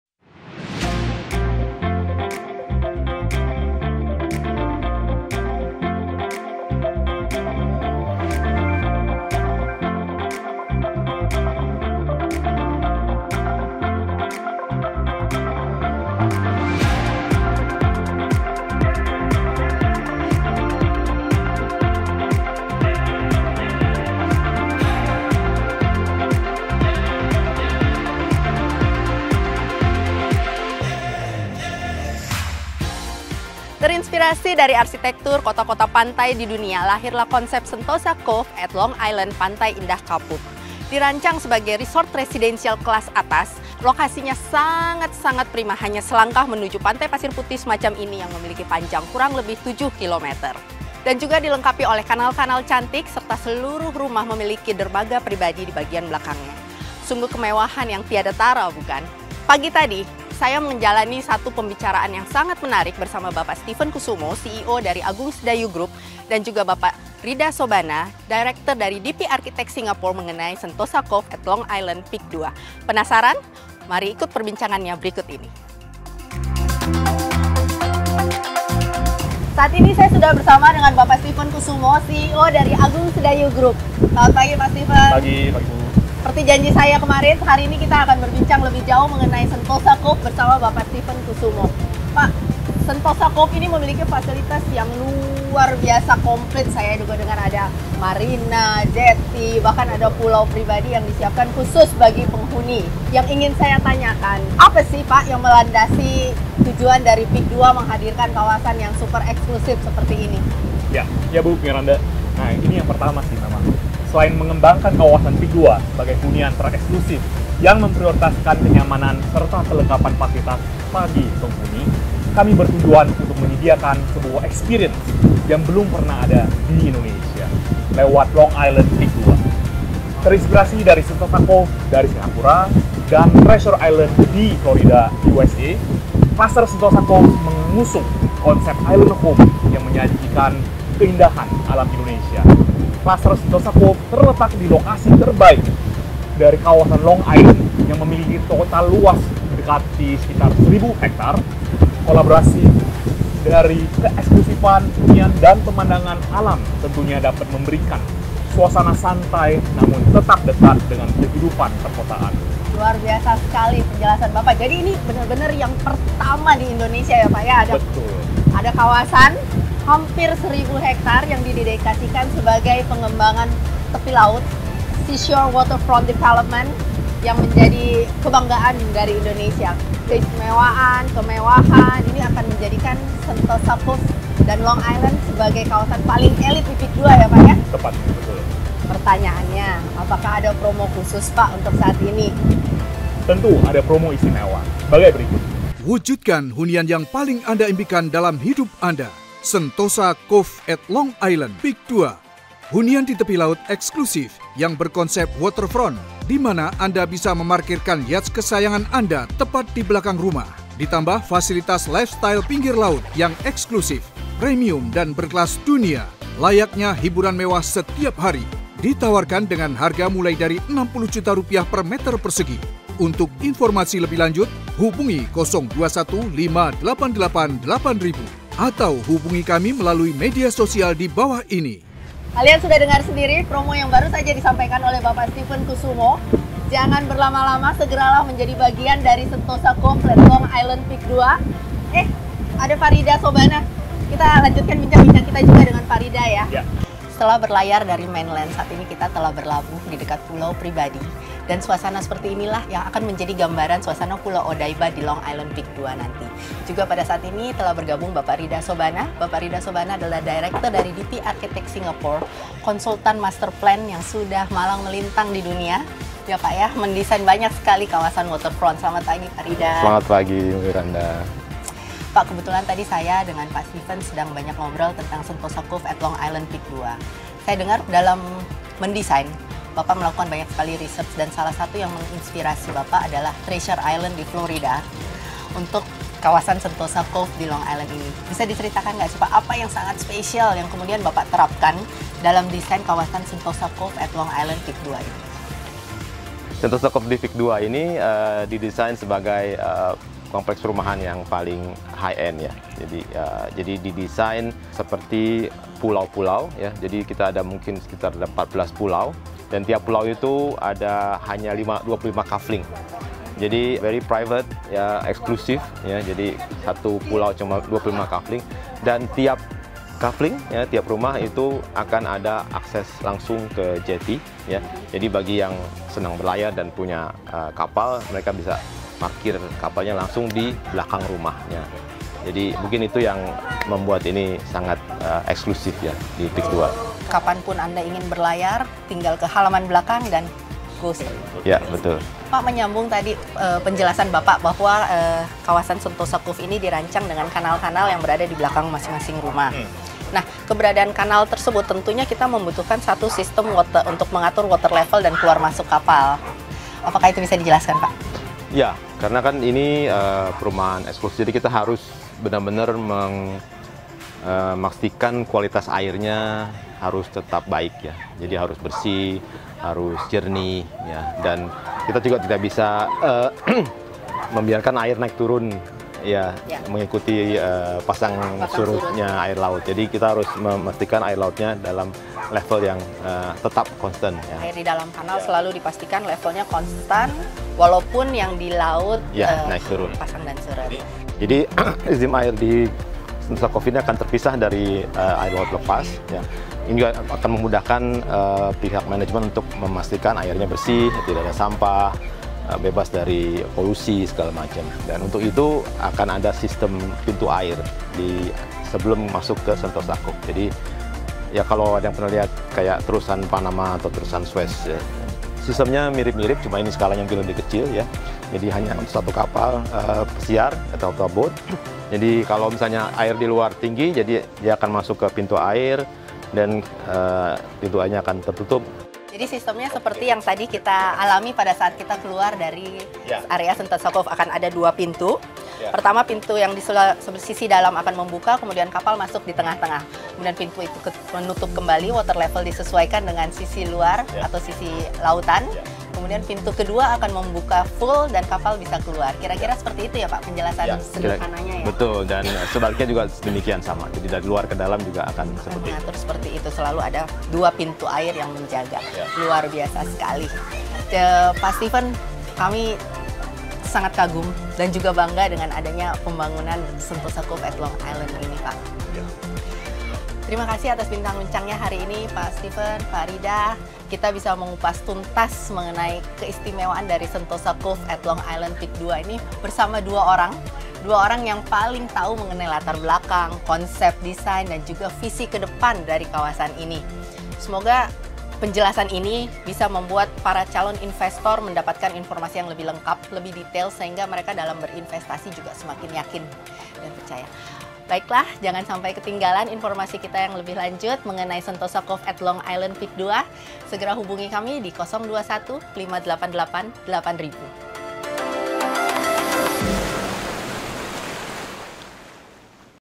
Bincang- Bincang Sentosa Cove PIK2 Bersama Bapak Steven Kusumo dan Bapak Rida Sobana
Halo SobatPIK2!

PIK2 berencana untuk membangun kawasan mewah tepi pantai yaitu Sentosa Cove PIK2 at Long Island.
Sebuah kawasan hunian berkonsep waterfront, dimana selain bisa memarkirkan mobil di depan rumah, Anda juga dapat memarkirkan kapal pribadi di belakang rumah.
Info Properti PIK2, hub:
Kunjungi Marketing Gallery PIK2:
Jl. Marina Indah Raya No.1 Pantai Indah Kapuk – Jakarta Utara.